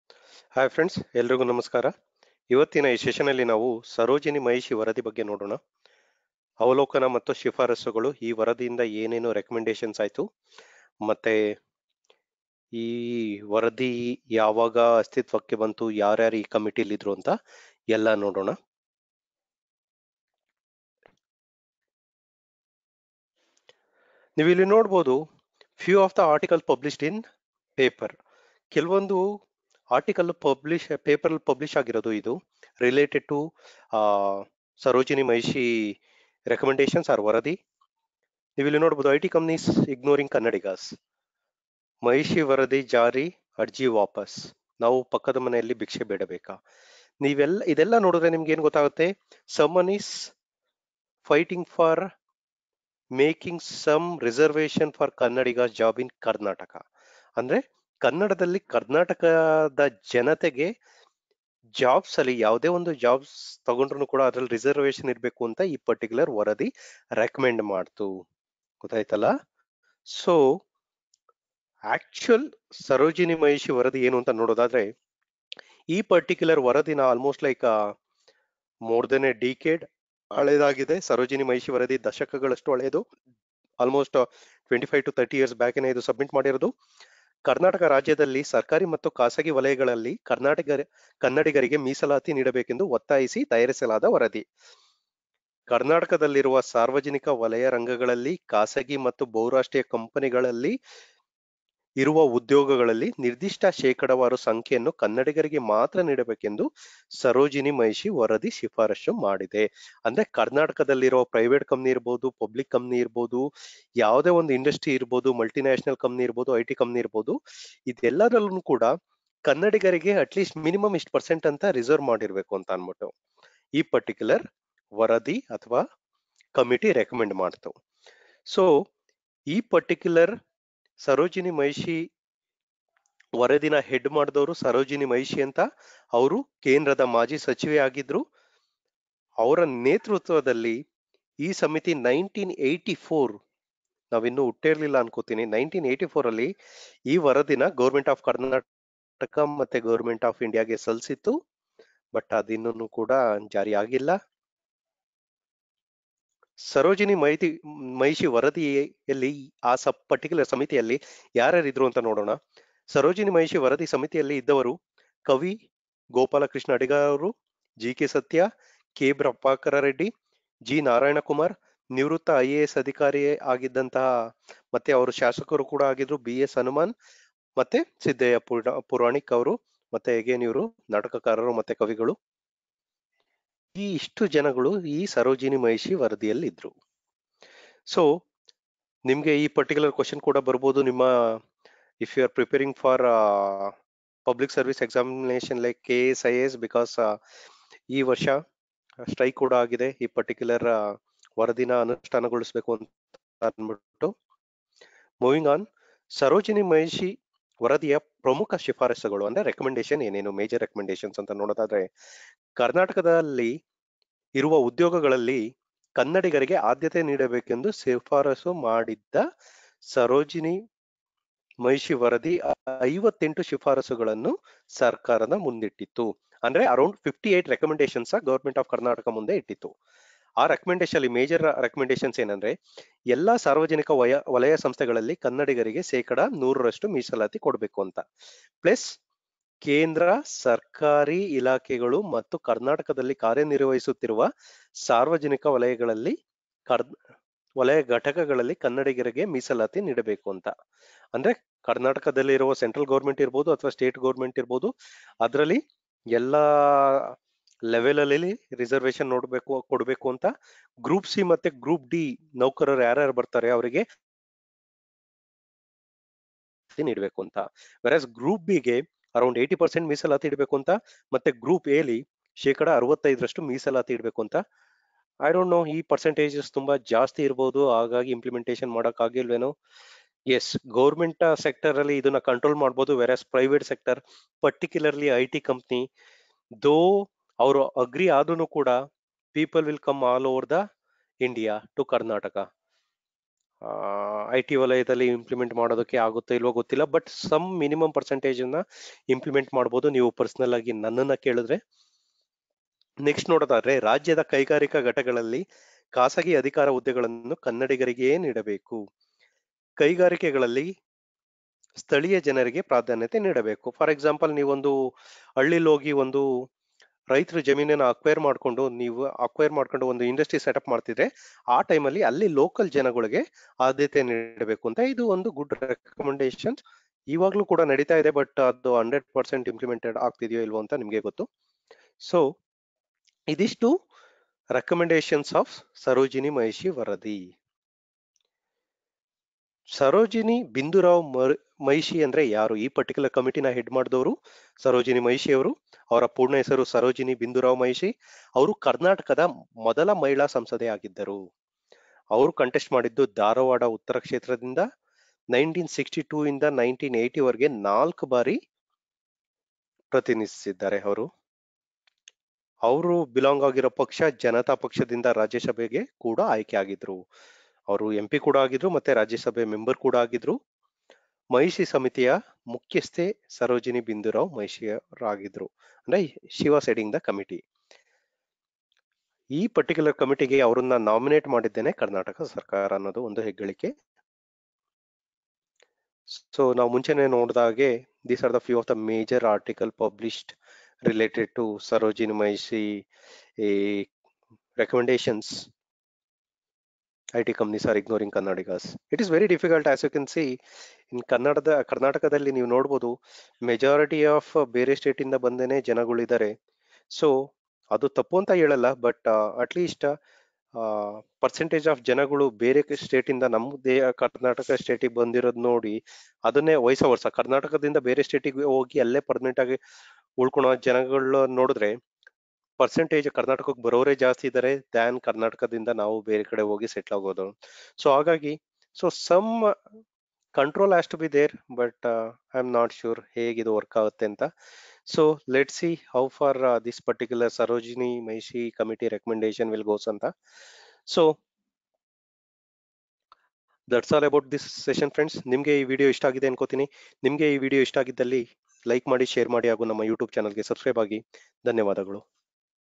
ಸರೋಜಿನಿ ಮಹಿಷಿ ವರದಿ : Sarojini Mahishi Committee Report & Recommendation
Watch this video to know about Sarojini Mahishi Committee Report & Recommendation.

# SarojiniMahishiCommitteeReport